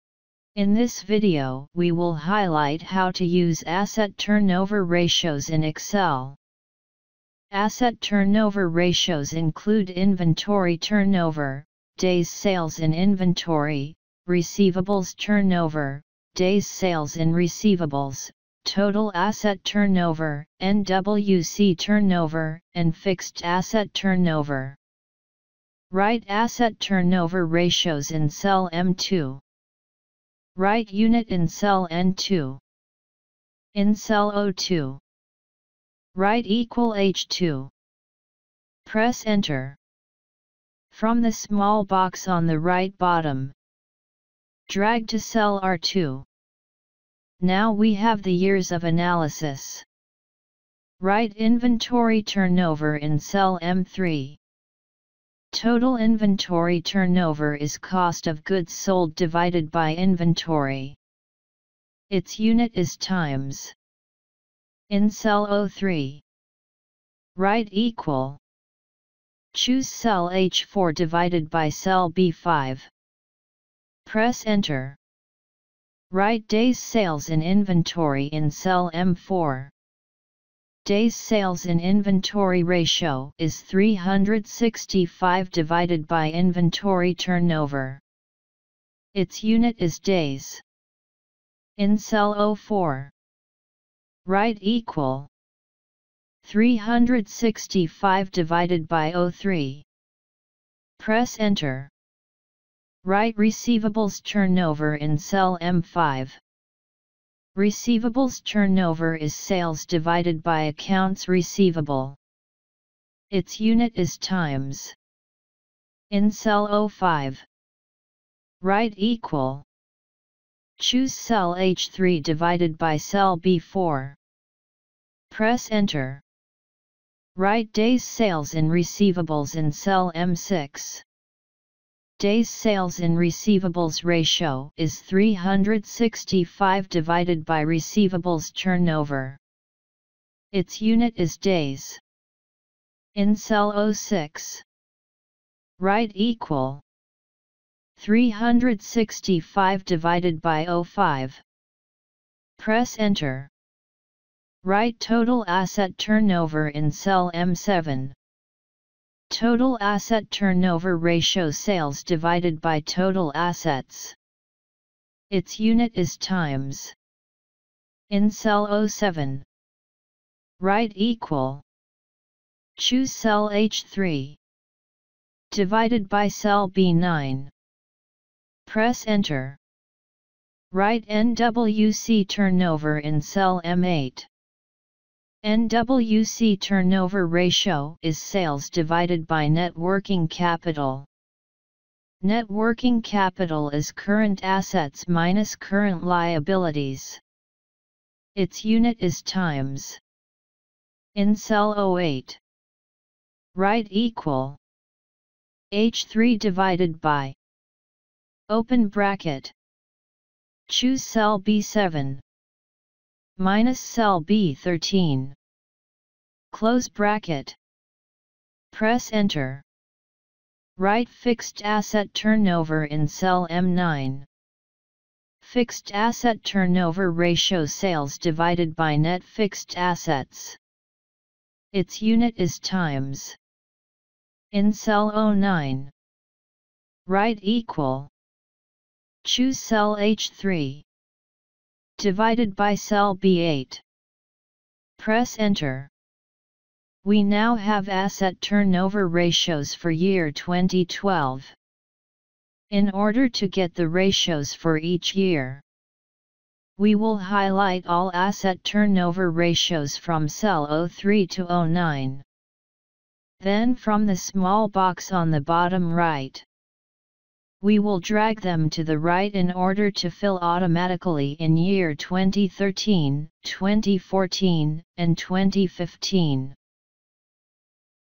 Asset Turnover Ratios
In this video we will highlight how to use Asset Turnover ratios in excel.